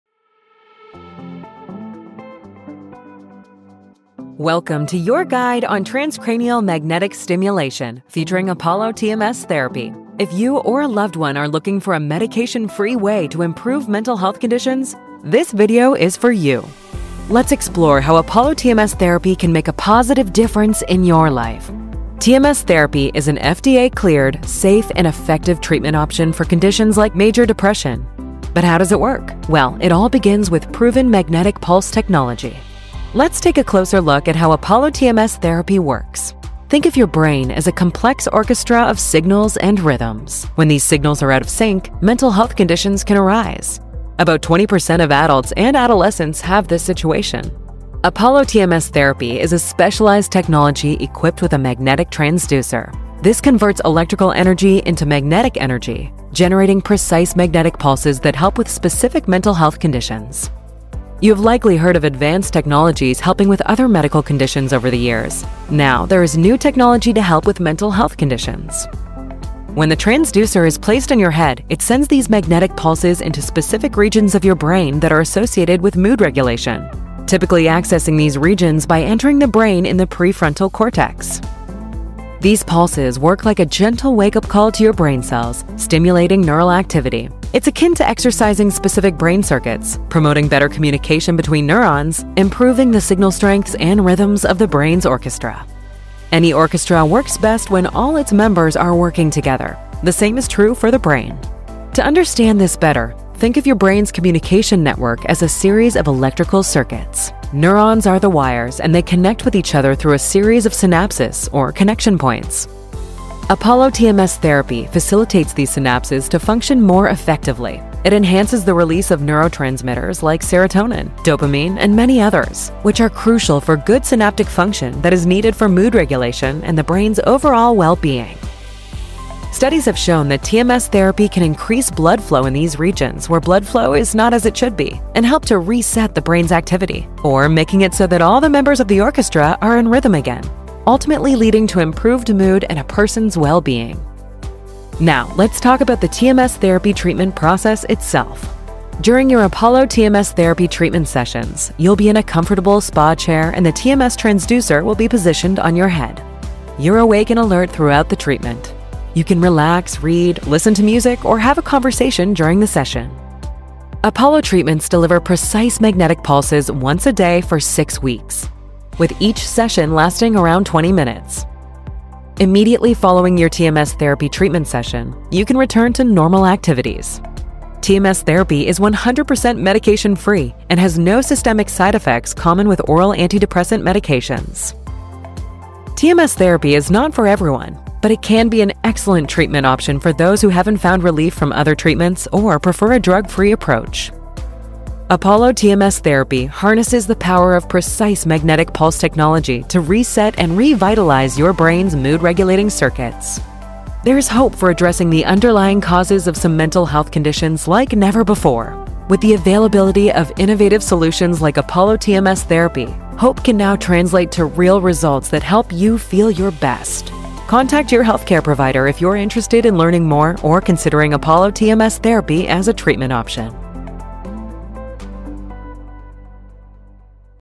How TMS Therapy Works: A Drug-Free Treatment for Depression
Struggling with depression and looking for a safe, effective, and medication-free treatment option? Discover how TMS Therapy works in this educational video.

TMS Therapy (Transcranial Magnetic Stimulation) is an FDA-cleared, non-invasive treatment that uses magnetic pulses to stimulate specific areas of the brain, helping improve mood regulation and mental well-being. This video explains how TMS Therapy works, who it can help, and what to expect during a session.

If you or a loved one are seeking new hope for managing depression, TMS Therapy may be the answer.

00:00 - Introduction: What Is TMS Therapy?
00:28 - Understanding Depression and Mental Health
01:00 - How TMS Therapy Works
01:45 - How TMS Targets Mood Regulation
02:20 - The Science Behind TMS Therapy
03:00 - What to Expect During TMS Treatment
03:40 - Benefits of TMS Therapy
04:15 - Is TMS Therapy Right for You?
04:50 - Learn More About TMS Therapy